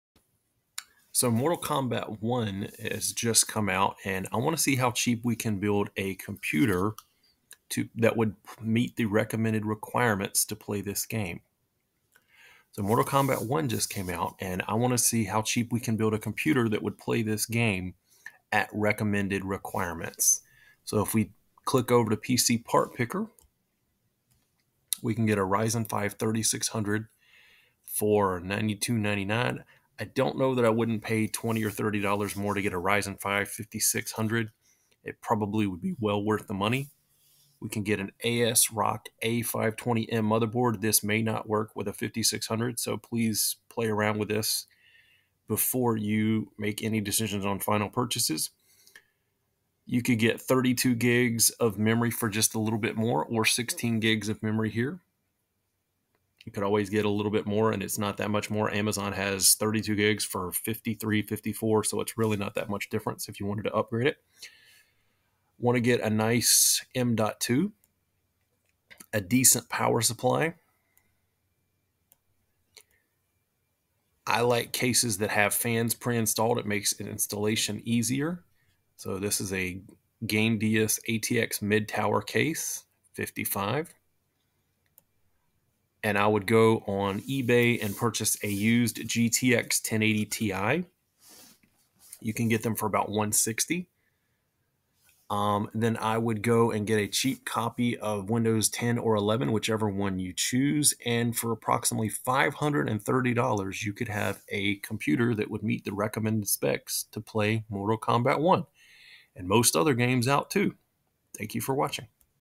Cheapest Gaming PC Series- Mortal Kombat Edition
In this video I go over the parts you would need to play  Mortal Kombat 1 at the recommended specs.  We are  back to the days of you being able to build a computer with the same price as the latest gen consoles with as much if not more power.  You can get a computer with the following specs for around 530.
GPU Recommend
Case Recommend